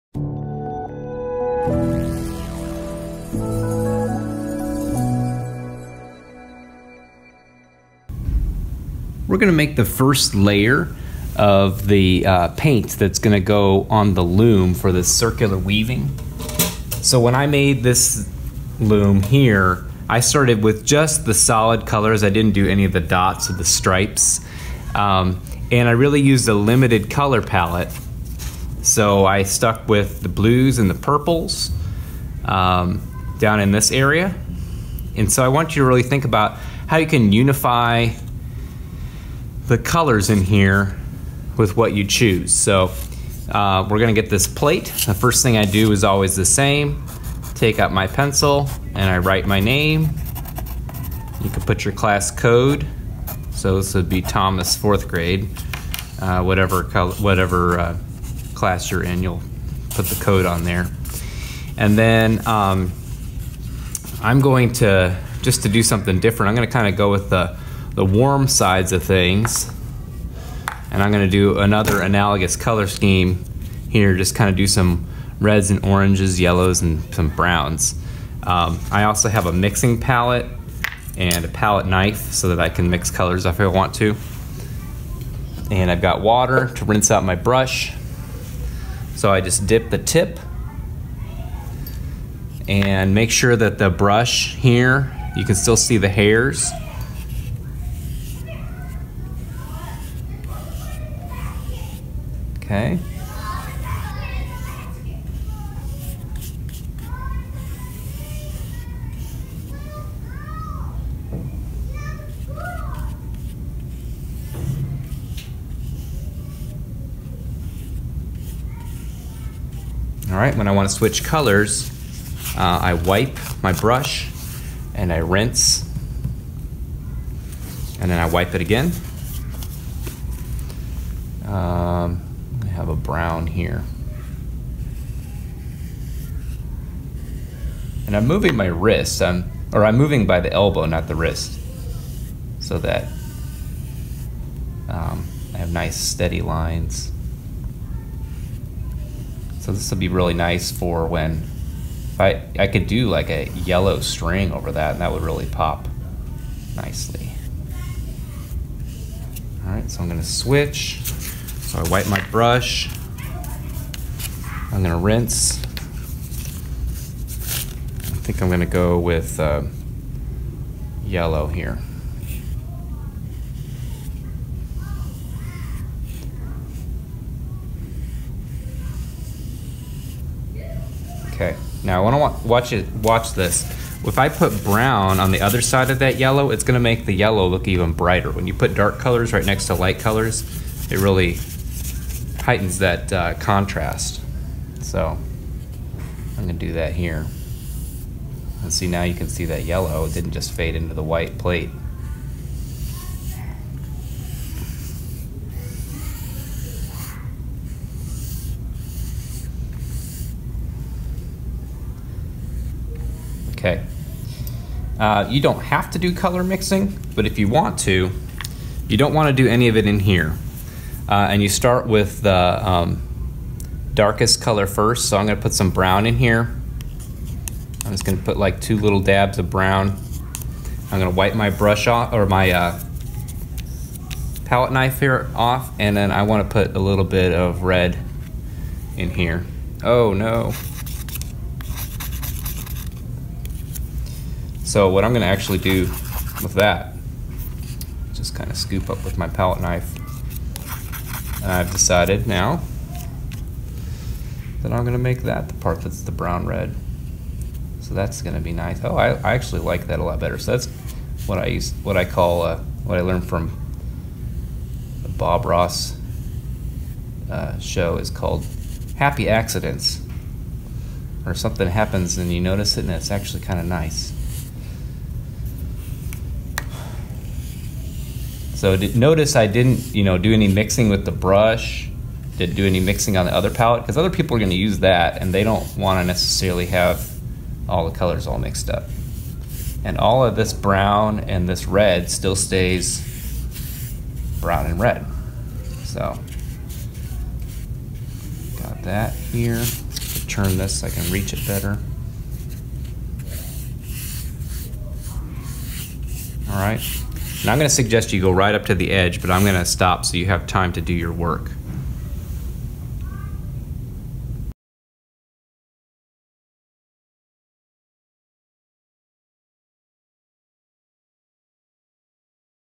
Circle Weaving - Painting the Loom Part 1
Painting a circle weaving loom with a limited palette